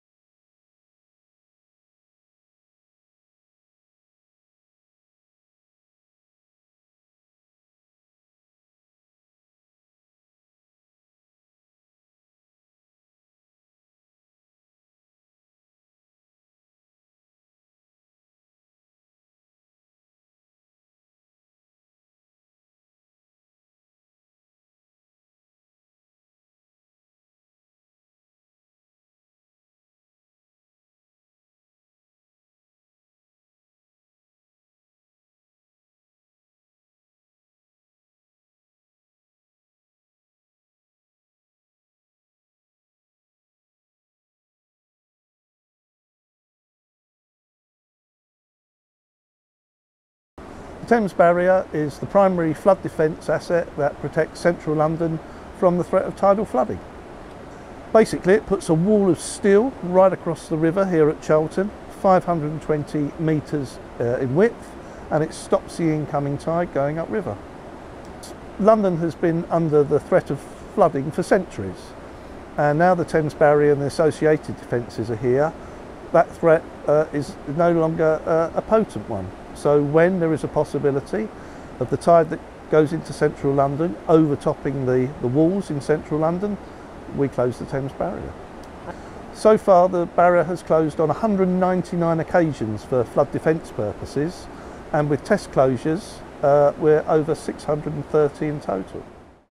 Thames Barrier shuts for annual test ahead of 200th operational closure
The Thames Barrier has been shut for its annual test closure, held at the start of each autumn to check equipment, systems and staff working on the flood defence system.

The Barrier's individual gates are to be moved into a range of operational positions, including fully closed, to stop the flow of the incoming tide.

First opened in 1984, the Barrier has been closed 199 times over its 37-year history.